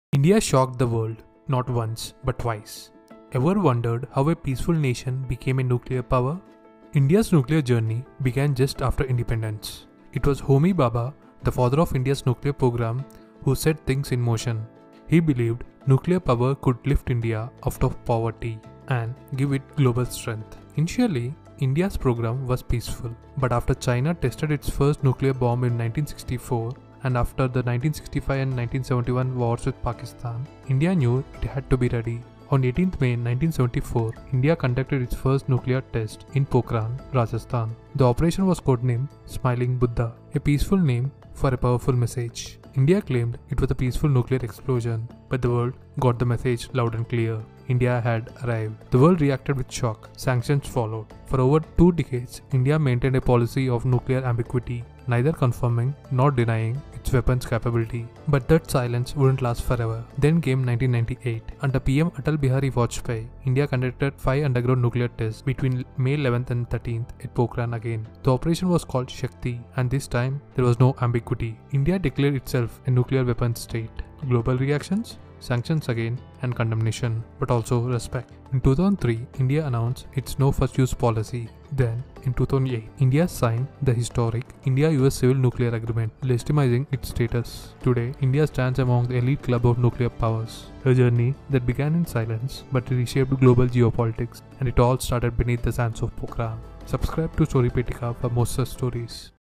How India Became a Nuclear Power? 🇮🇳💥☢️
In the deserts of Pokhran, history was made. Against global odds and under utmost secrecy, India conducted its first nuclear test in 1974—code-named Smiling Buddha. Decades later, in 1998, India boldly reaffirmed its nuclear capabilities with Operation Shakti. This is the gripping story of how India became a nuclear power—driven by science, strategy, and sovereignty.